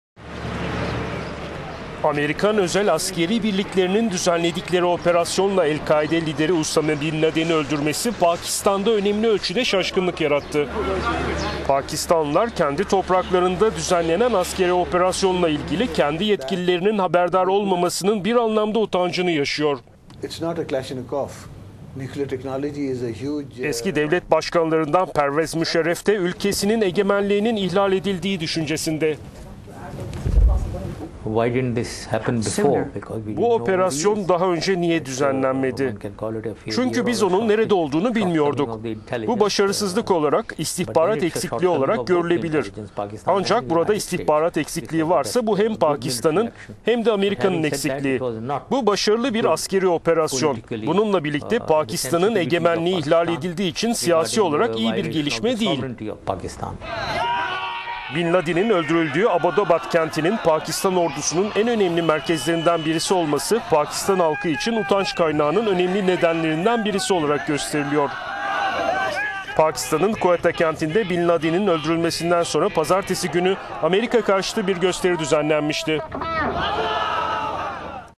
ABD operasyonu Pakistan'ı utandırdı
Amerikan özel askeri birliklerinin, düzenledikleri operasyonla, El Kaide lideri Usame bin Ladin'i öldürmesi, Pakistan'da önemli ölçüde şaşkınlık yarattı.

Pakistanlılar, kendi topraklarında düzenlenenen askeri  operasyonla ilgili, yine kendi yetkililerinin haberdar olmamasının bir anlamda utancını yaşıyor.